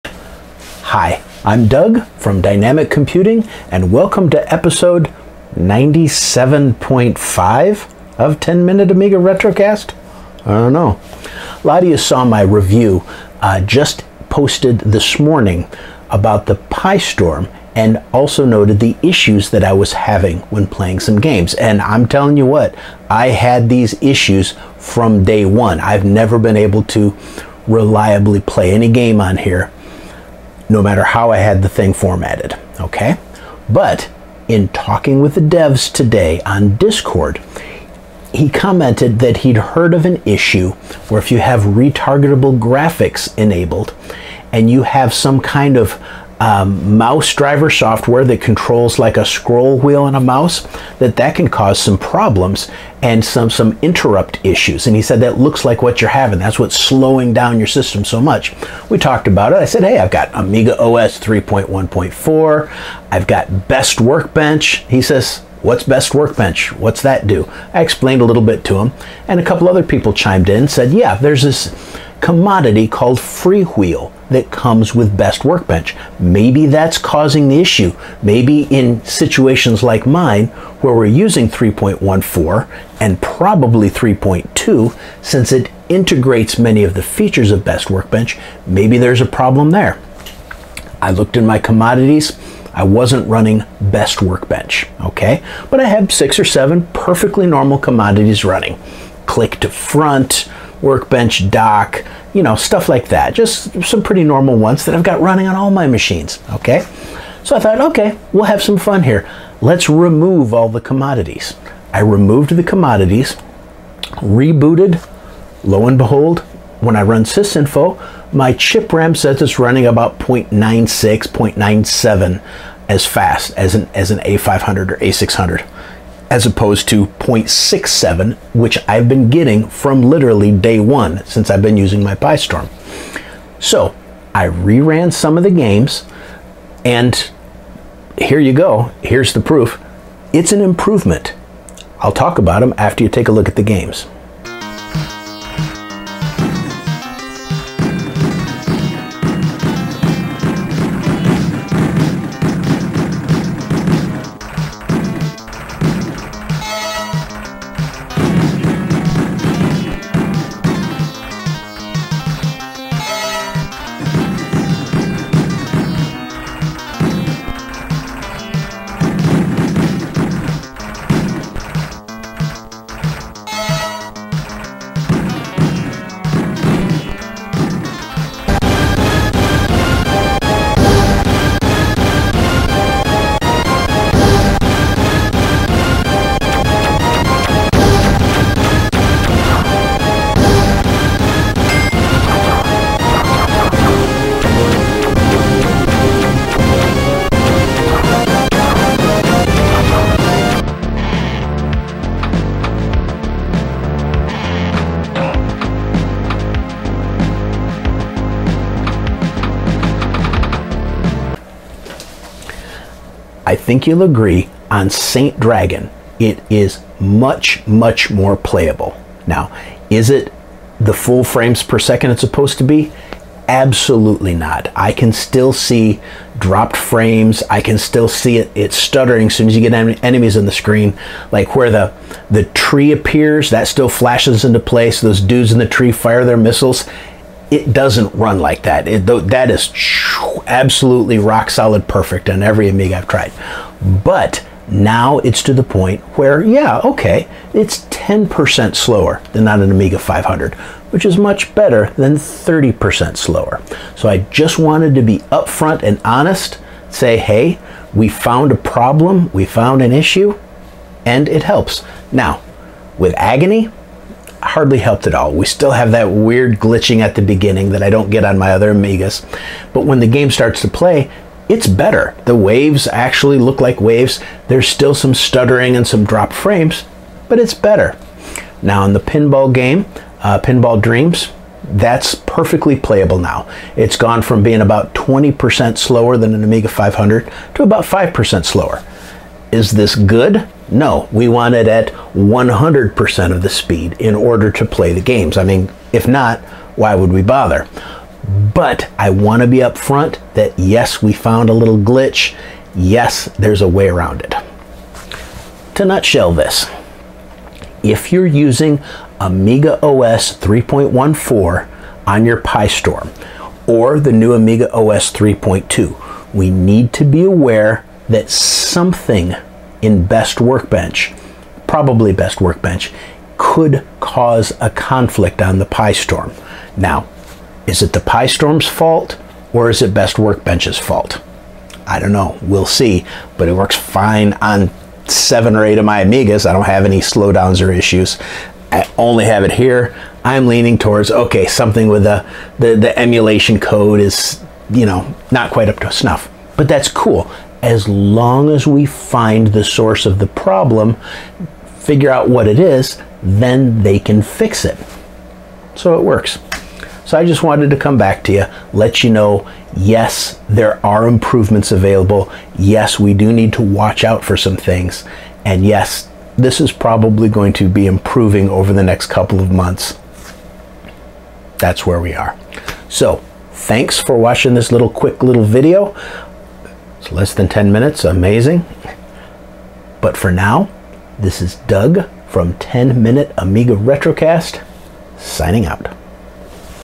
Addendum to the PiStorm Review - My CHIP RAM is faster now!
Well, after speaking with the Devs about my CHIP RAM speed issues, it appears it is something with BestWB on Amiga OS 3.1.4 - when I disabled all my commodities, the CHIP RAM performance jumped up substantially - and to my surprise, if I re-enable the commodities... every thing is fine! Huh?